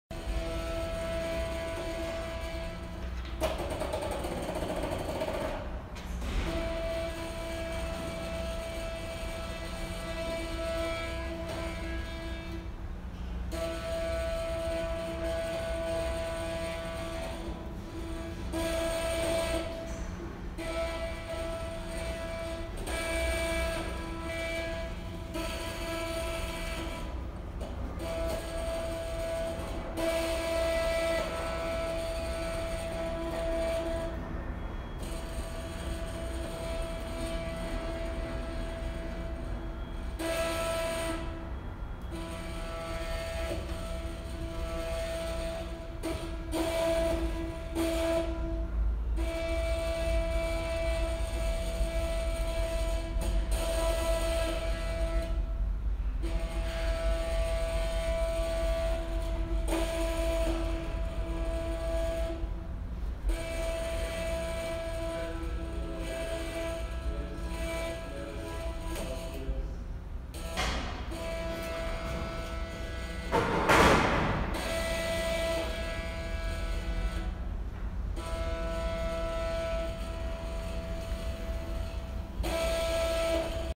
Public Installation Artwork by Dasol Hong. "Trans:layer" (2019)
Installaed at Pfizer Building, Brooklyn, New York.
Dasol Hong. "Trans:layer" (2019). Nylon mesh, plaster, water, stepper motor, rubber, fabric, 40 × 70 in